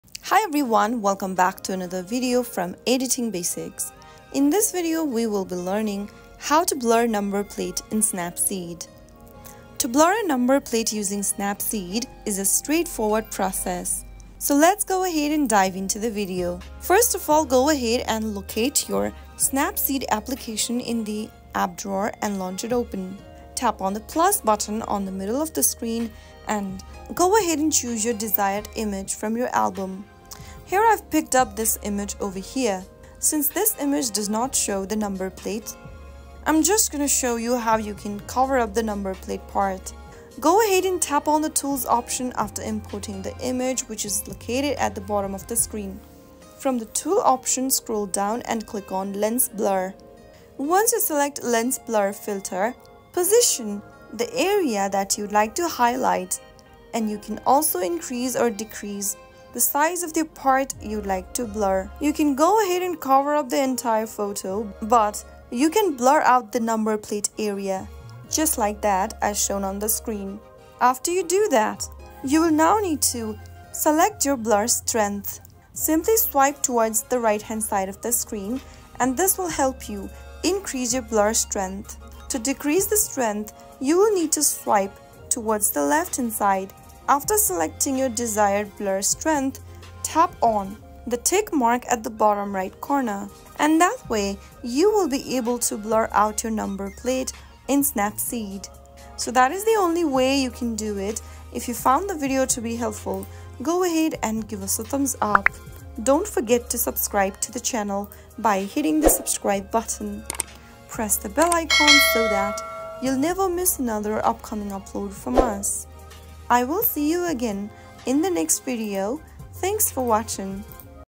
How to Blur Number Plate in Snapseed 2024?
In this tutorial, we'll show you how to blur a number plate in Snapseed, a powerful and user-friendly photo editing app. Blurring number plates is essential for protecting privacy or complying with regulations when sharing photos.

Blurring number plates in Snapseed is straightforward and ensures you can safely share your photos while protecting sensitive information. We'll guide you through each step to achieve professional results with ease.

Feel free to customize the description based on specific features of Snapseed and your audience's preferences.